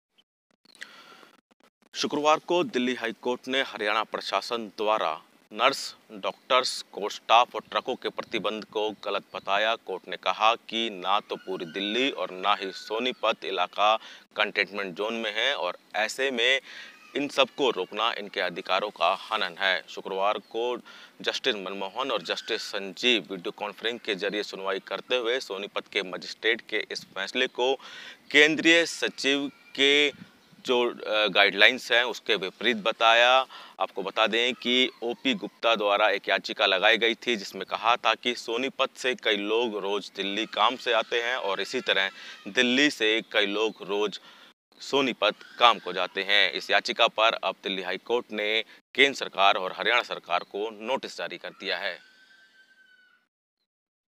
Delhi HC ने सोनीपत में एंट्री रोकने पर नाराजगी के साथ उठाया ये कदम ?
DelhiHC ने सोनीपत में एंट्री रोकने पर नाराजगी के साथ उठाया ये कदम ?